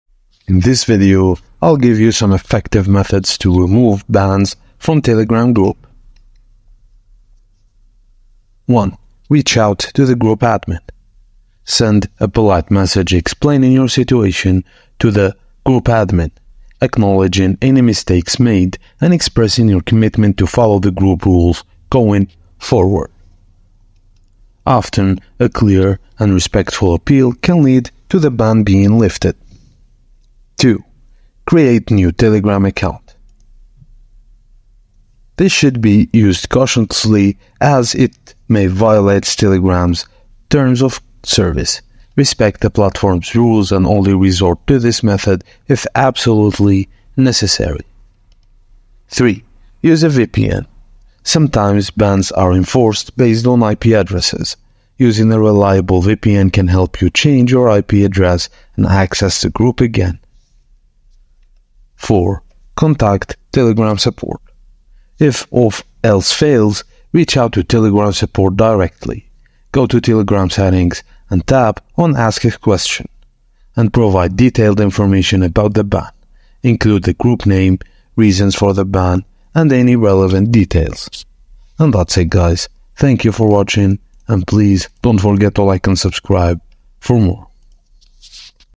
How to remove a Ban from a Telegram Group❗(2024) (Get Unbanned fast)✅
🎥 In this video I'll show you How to get unbanned from telegram group?. The method is very simple and clearly described in the video. 🍳 Follow all of the steps & Can't access the chat | Sorry, you can't access this chat because you were banned by an admin

00:00.Intro
00:05.How to remove a Ban from a Telegram Group
01:00.outro